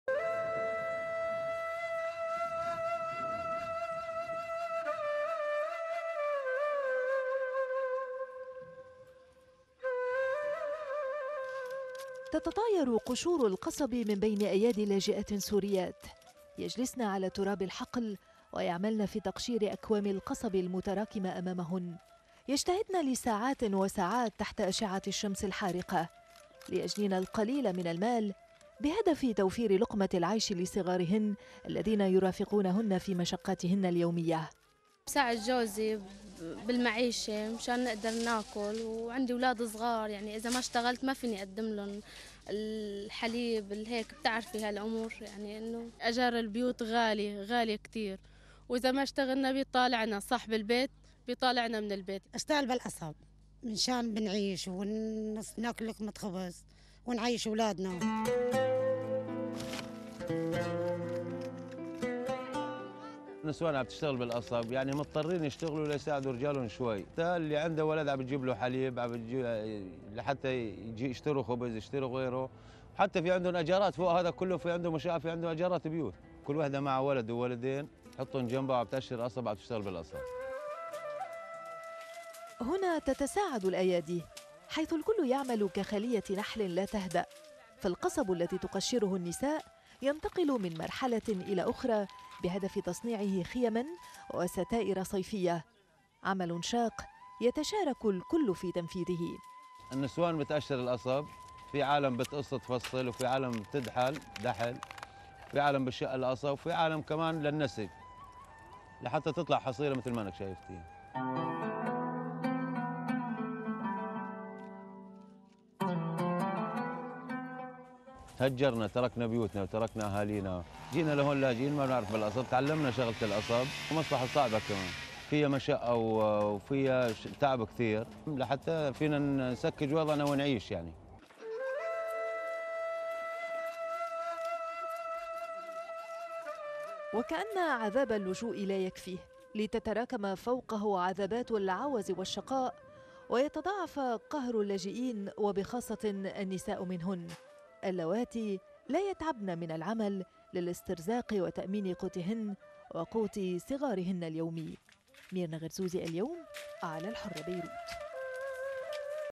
لاجئات... وكادحات
بحثًا عن لقمة العيش... سوريات يعملن في تقشير القصب